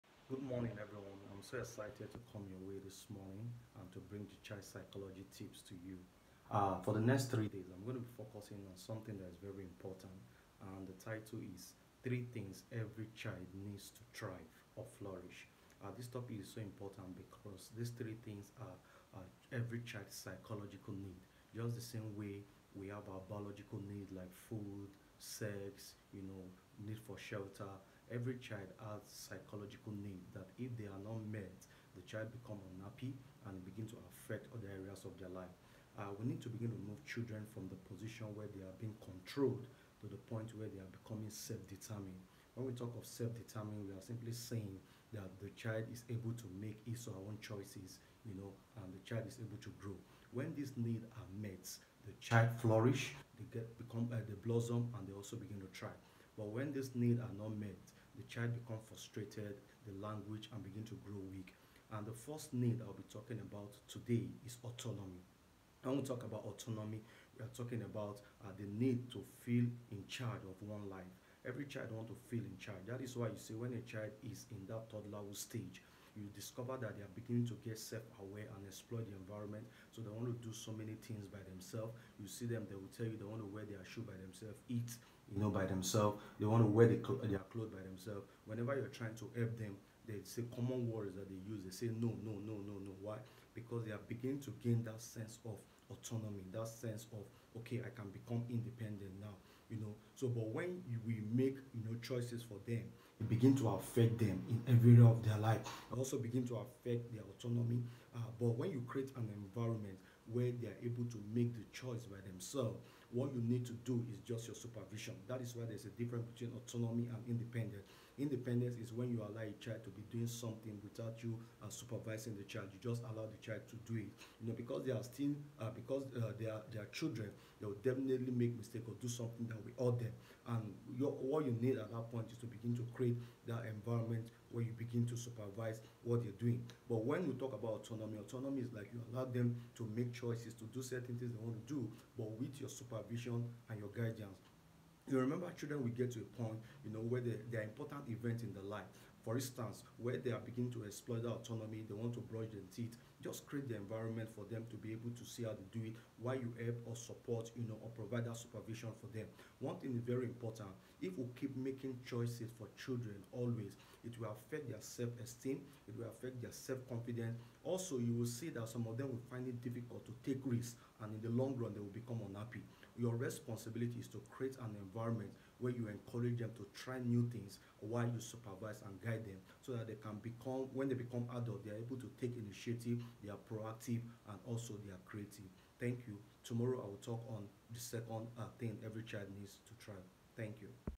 Three things every child needs to thrive and flourish
There are three psychological needs every child needs to thrive and flourish. Just as our biological needs(food, sex, etc) are important so are the psychological needs. In this video my focus is on the first need which is AUTONOMY.